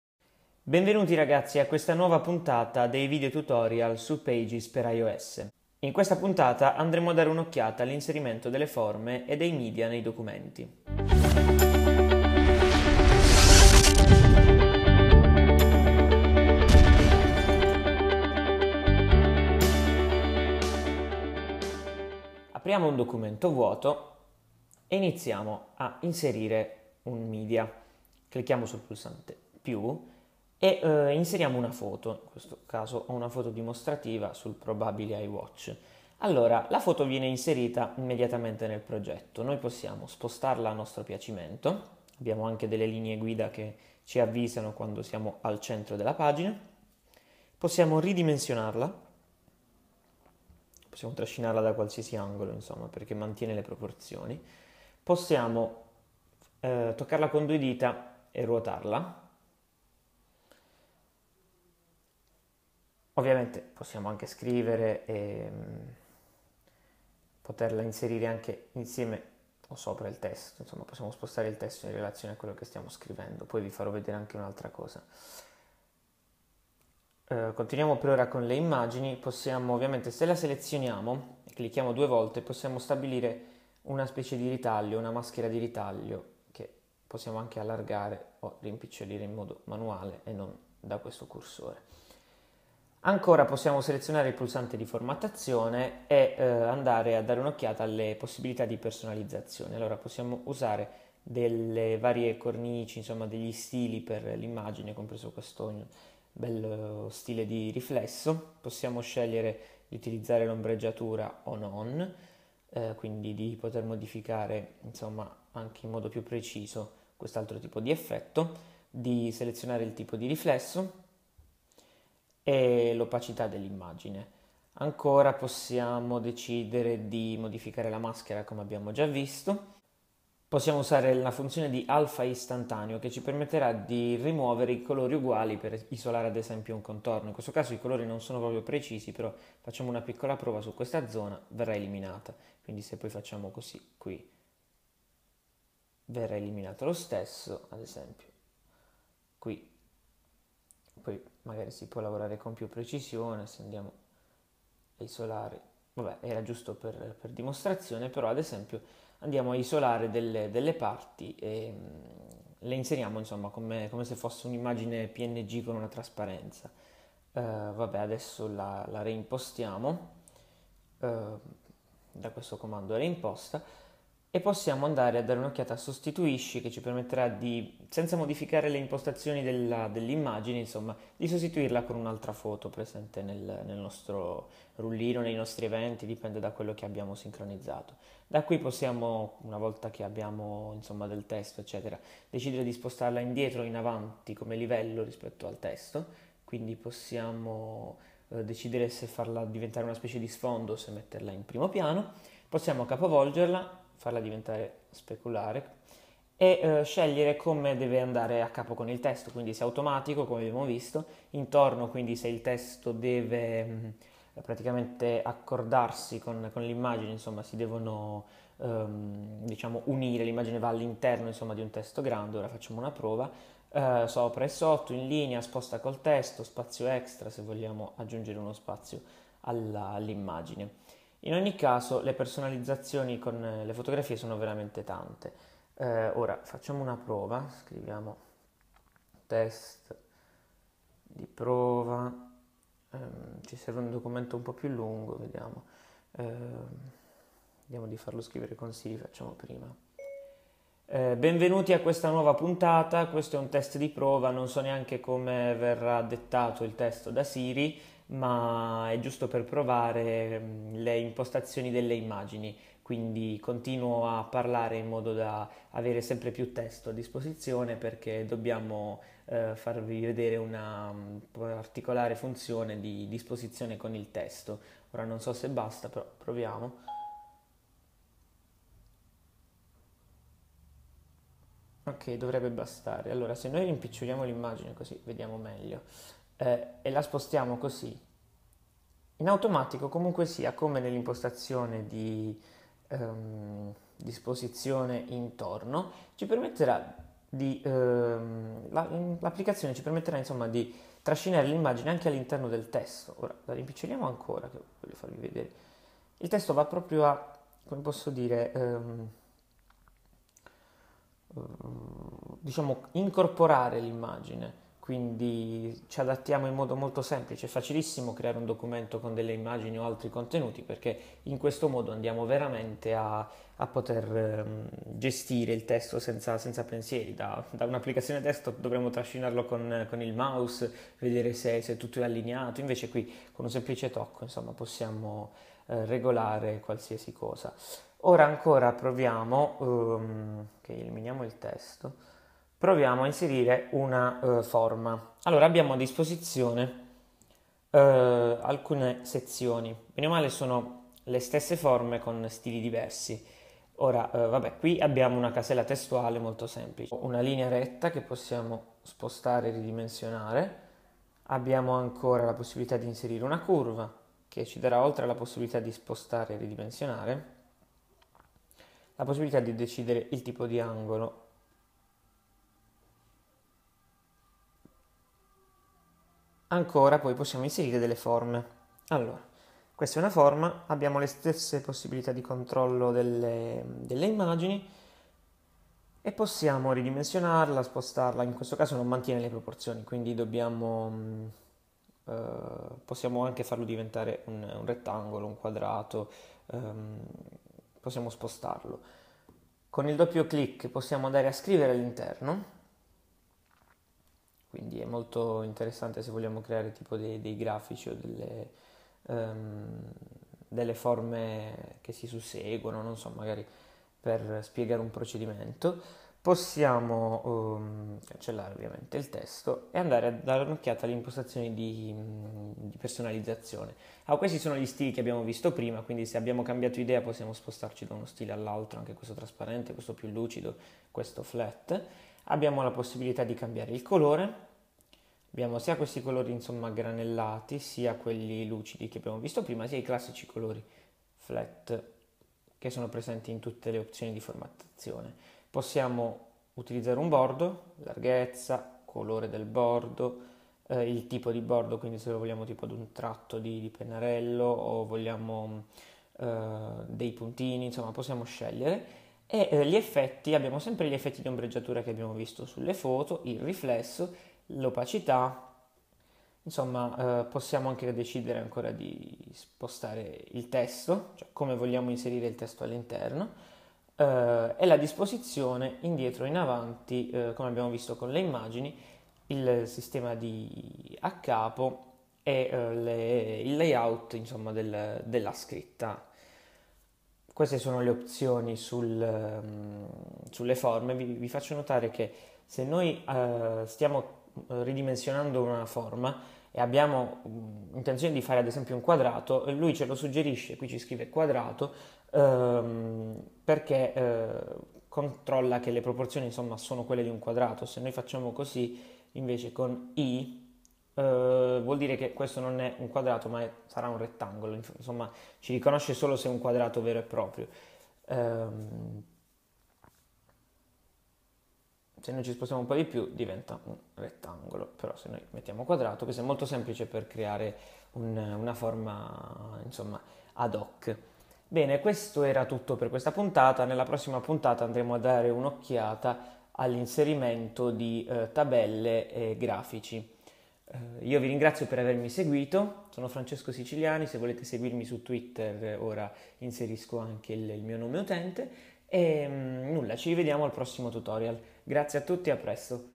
Tutorial Pages iOS - #3 (Forme e Media)
Ecco il terzo di una serie di video tutorial sull'utilizzo dell'applicazione per l'elaborazione di testi offerta da Apple, Pages.

Here's a video tutorial for Pages on iPad. This app is a complete text creator made by Apple.